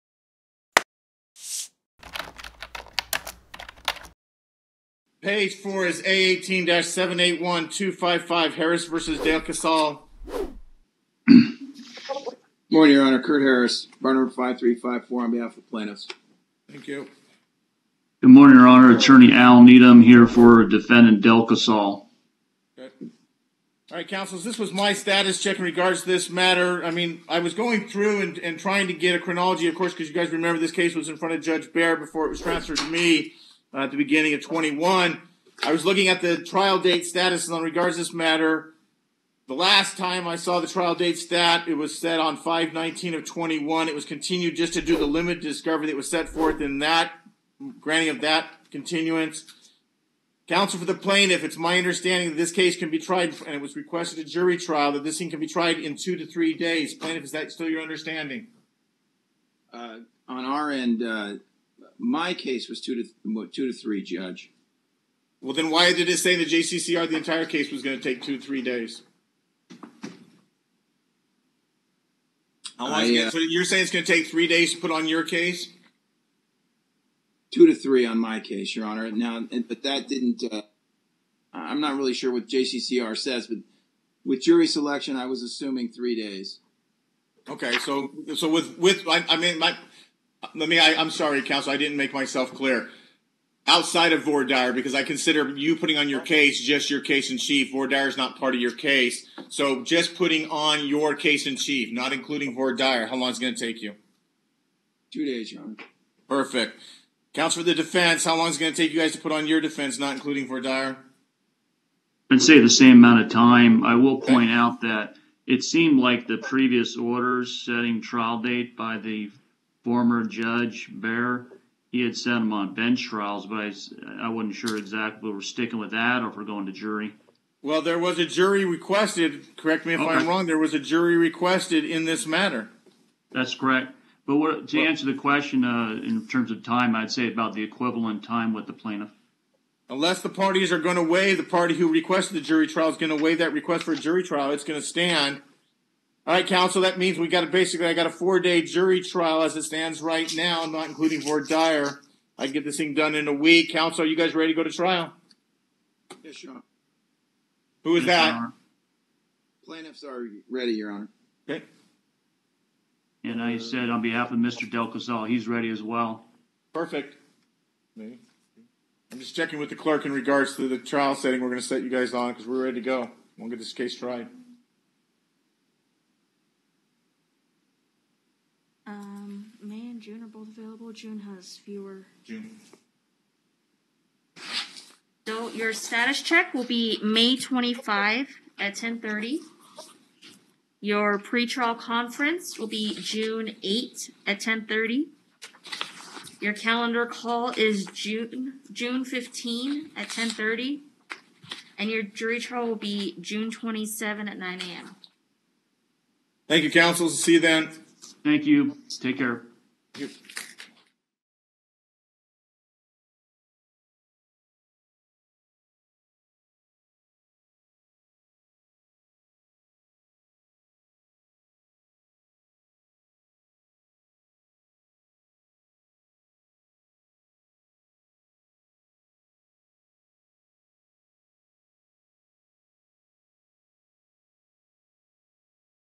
Kurt Harris vs. Ernest Del Casal, February 8, 2022
The Honorable David Jones, Eighth Judicial District Court, Department 29, presiding over a status check. Attorney Kurt Harris appeared in proper person. Attorney Al Needham appeared on behalf of Ernest Del Casal. Case No. A-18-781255-C. The Court schedules a jury trial.